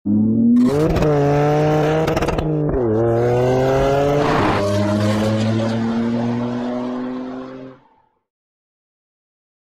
Collecting Cars GR Yaris - FullThrottle
Check out the full video to find out more about our new Yaris GR and what we have in store for it.